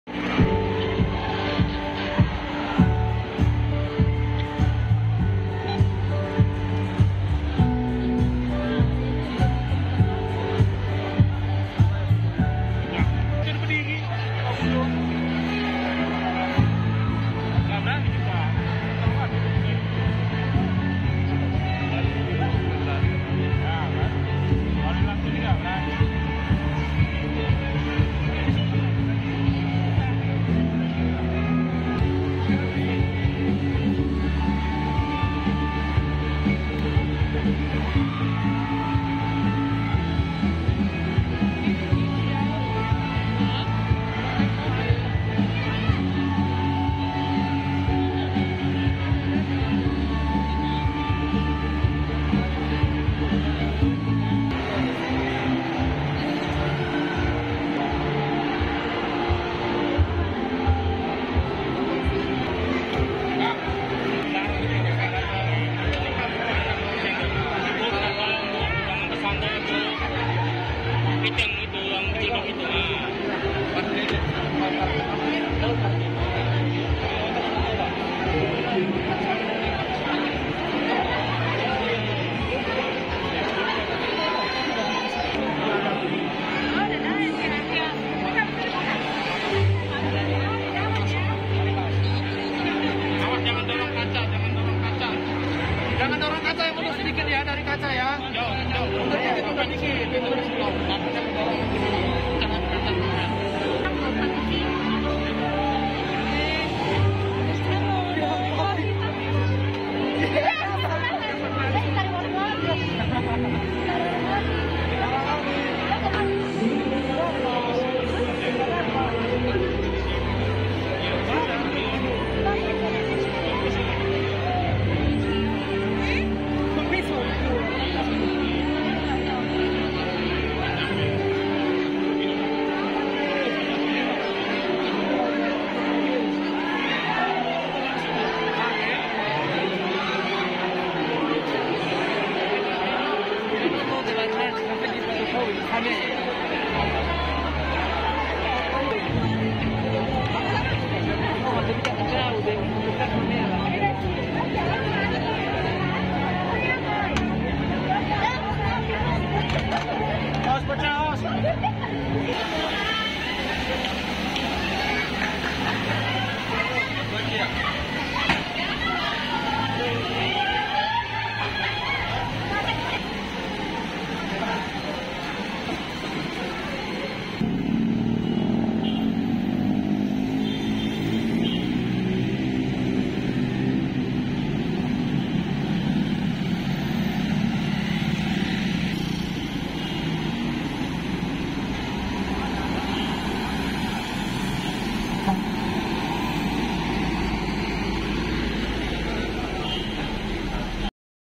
Kunjungan Jokowi Ke Sun Plaza Medan
Pak president Jokowi beserta rombongan tiba di tempat perbelanja an Sun Plaza Medan,warga antusias menyambut kedatangan nya ke Kota Medan

Pengawalan ketat oleh Paspampres di sekeliling Gedung Sun Plaza Medan

Pak jokowi membagikan baju baju kaos untuk masyarakat yang menyambutnya